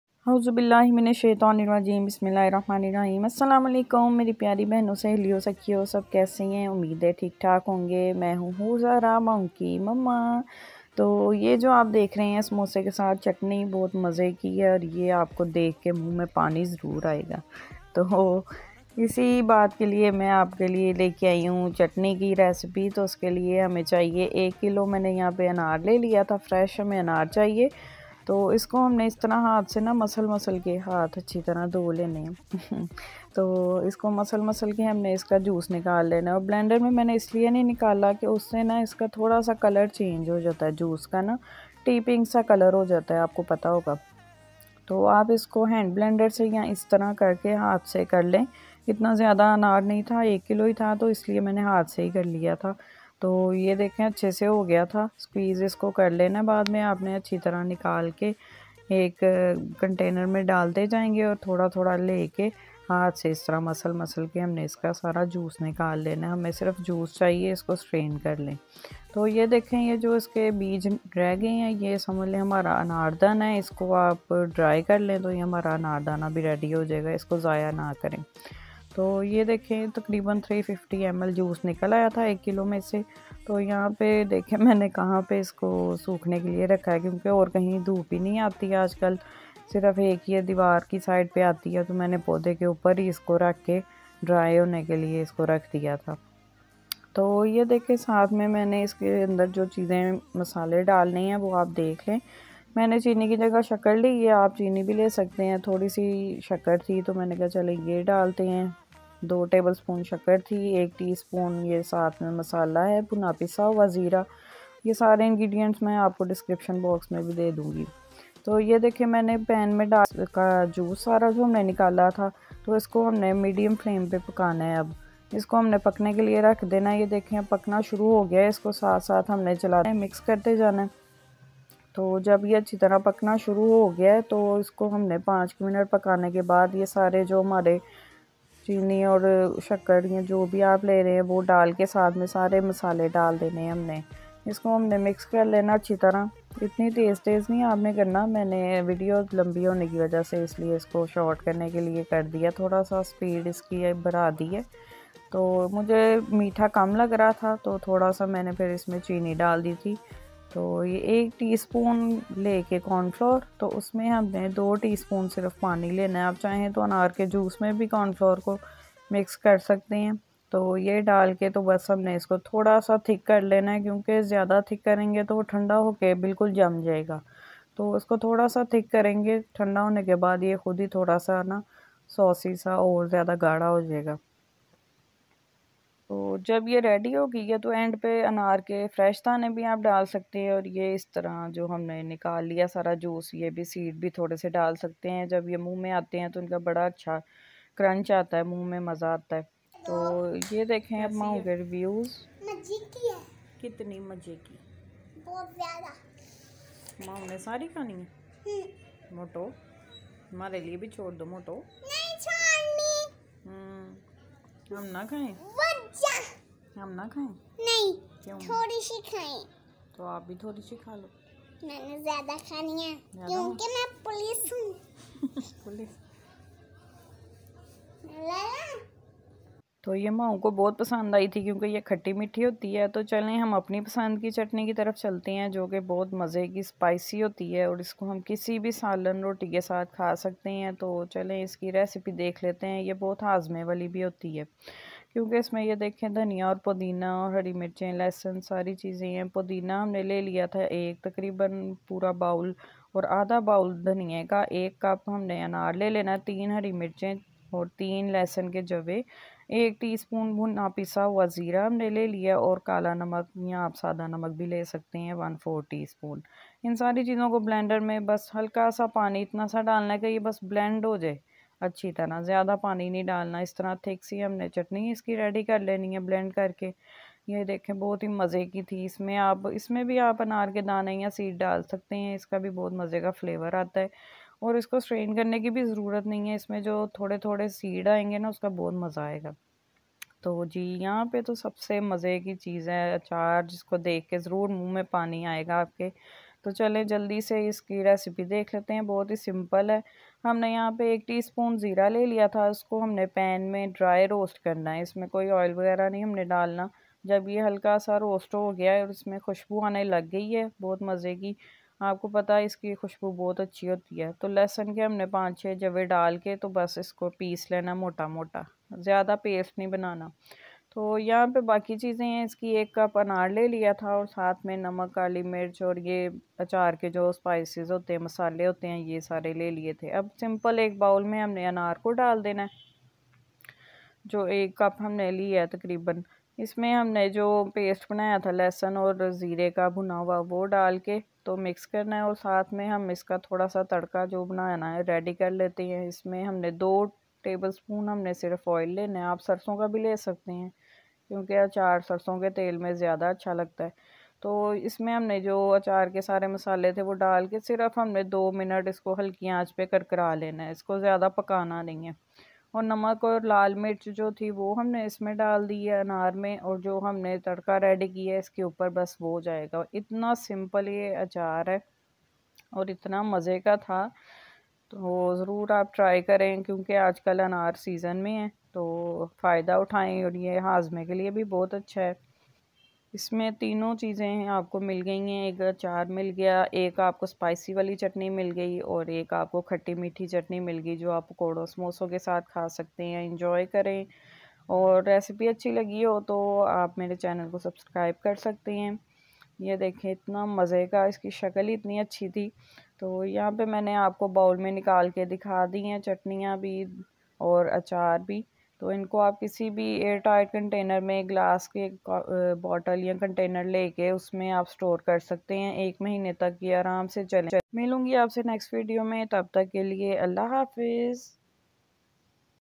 Anaar Ka Achaar or 2 Types Of Chutney 😋5 Mints me Ready||Maun ki Mama ♥️
Assalamualaikum All...umeed h sb kheryt se hongy ..Is video me Anaar jo k Ajkl Season me hy...uski bht different recipes bnai gai hn...jin me AnarKaAchar KhattiMeethiChatni SpicyChatni bnai hn...or besttt taste h inka aik bar try zrur kren or mazeed mzydar videos k lye keep watching Maun ki Mama ♥️
JazakAllah ☺️